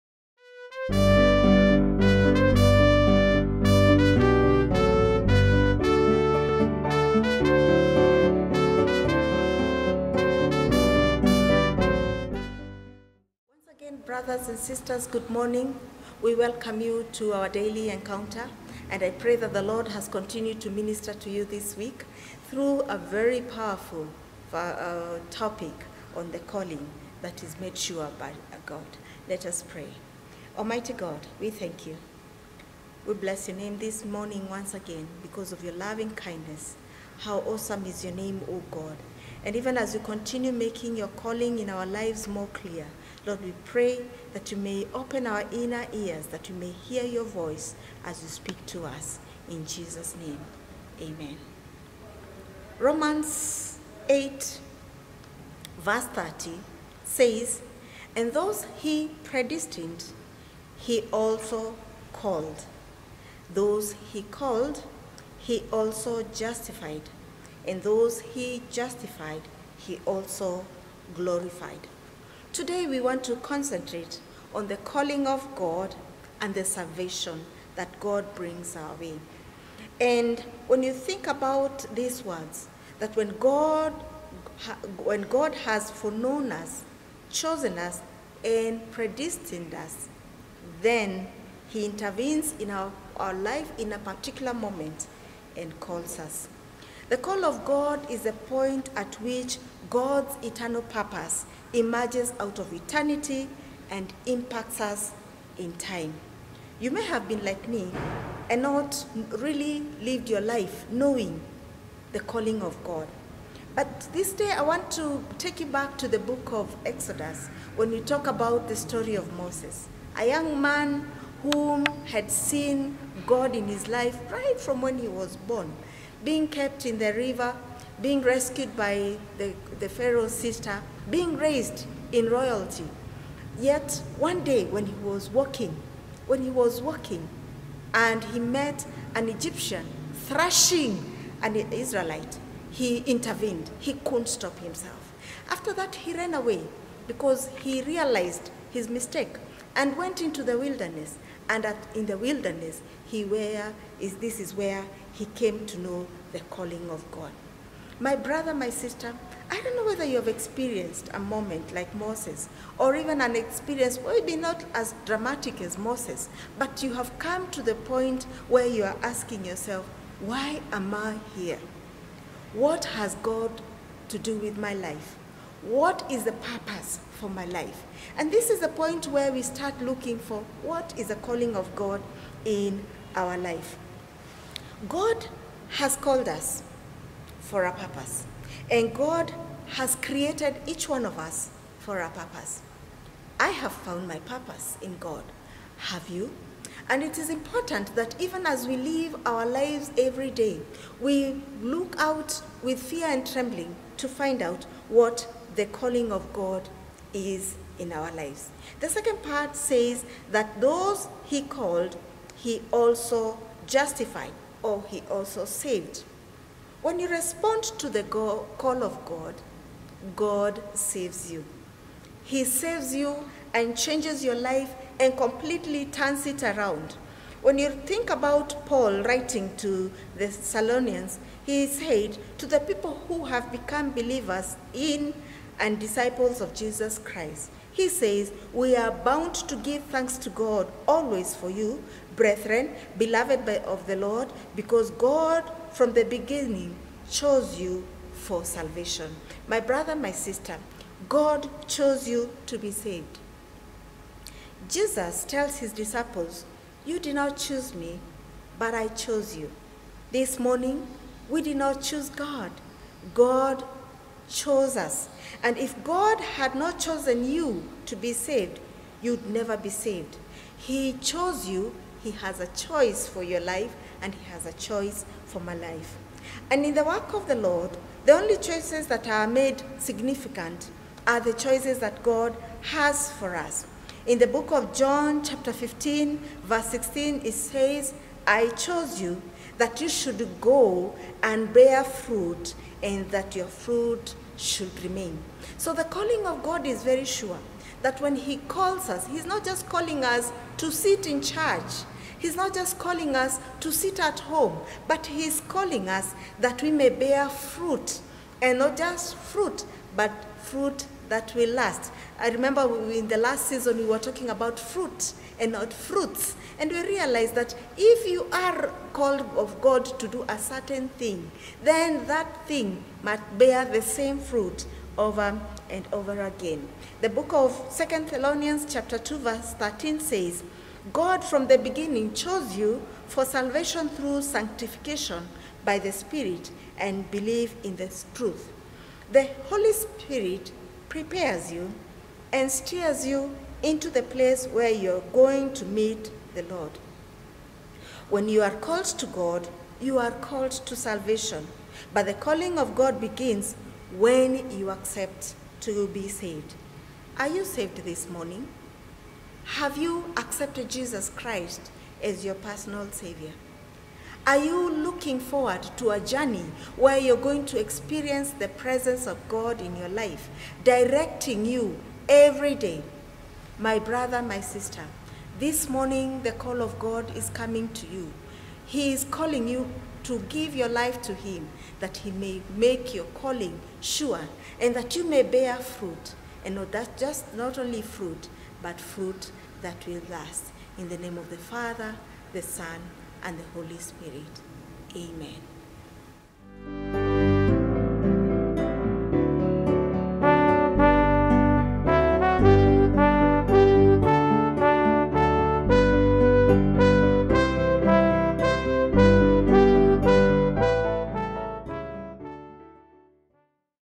YOUR DAILY ENCOUNTER
30TH OCTOBER 2020
"The true God has no beginning and has no end, it was not begotten and cannot beget, it cannot die and resurrect. He is responsible of everything good and evil and he is the sustainer of life. If he can save he saves all without distinction. "
Join our Lay Reader Hellen Njuguna as she  take us through the topic 'He Calls Us and He Saves Us.'
Prayer Lines: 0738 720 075 / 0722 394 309 / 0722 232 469 / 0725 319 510